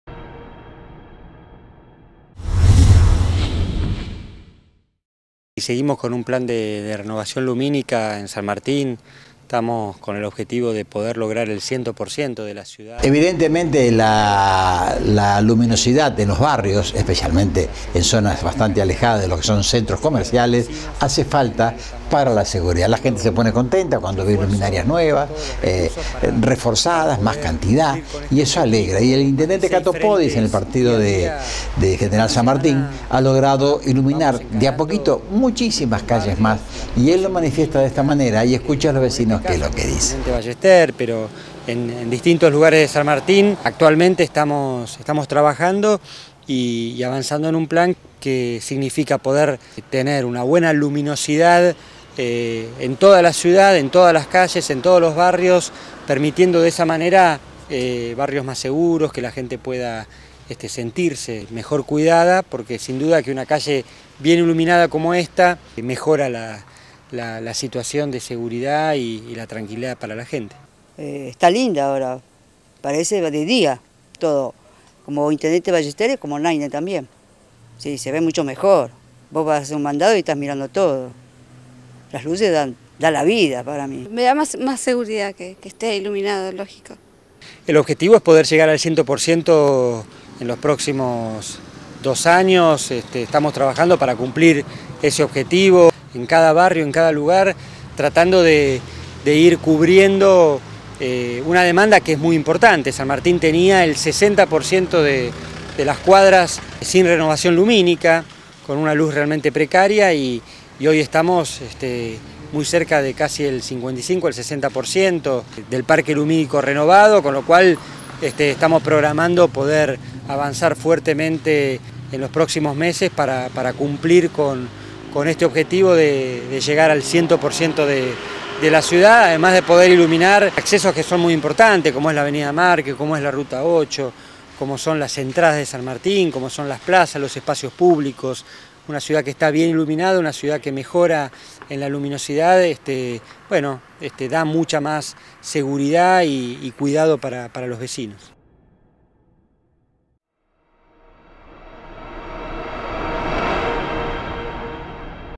Hacemos Comentarios Tv - San Martin Ilumina su Ciudad
La iluminación es parte del plan de seguridad que San Martin desarrolla en beneficio y tranquilidad de los vecinos, quienes comentan de sus ventajas.